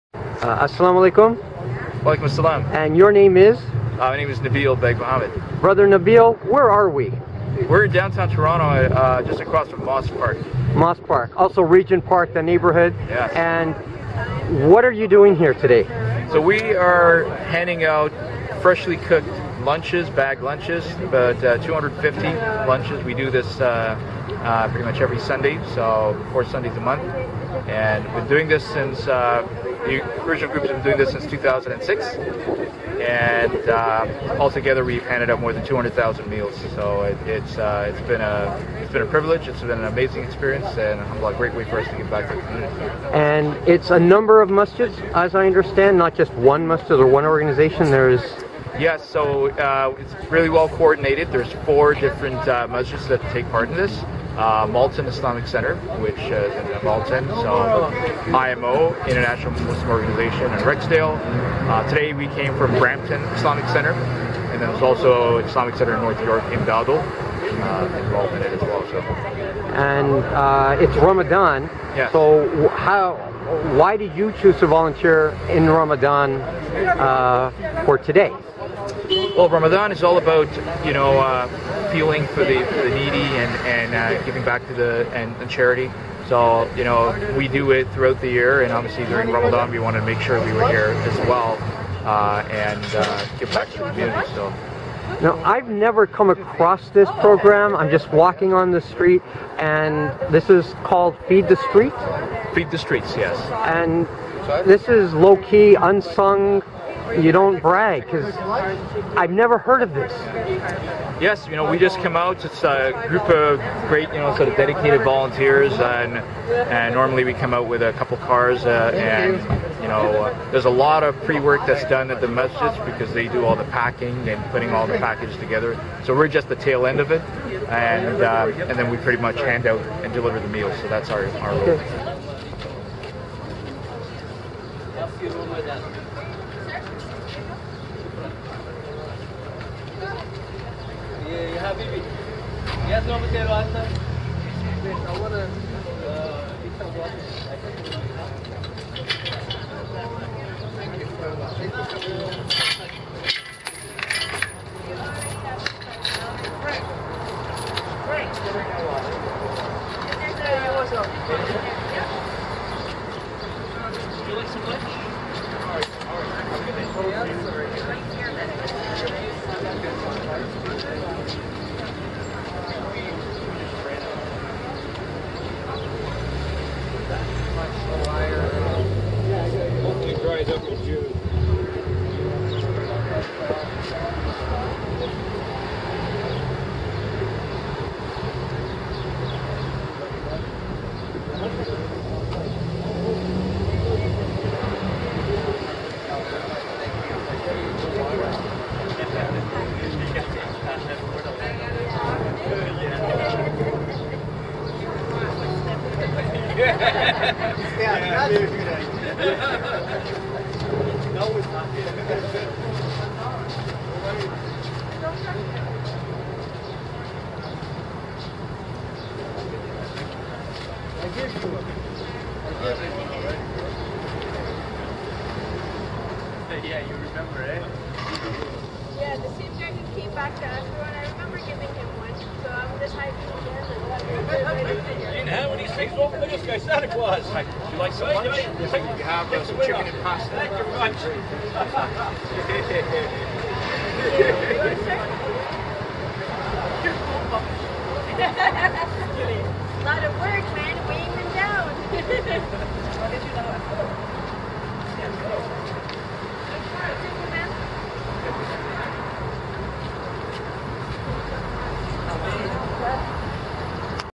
HiMY SYeD -- Brother Nabil Muhammad, Feed The Streets, Sherbourne Street, Toronto, May 26 2019
HiMY SYeD -- Brother Nabil Muhammad, Feed The Streets, Sherbourne Street, Moss Park, Downtown Toronto, Ontario, Canada, Sunday May 26 2019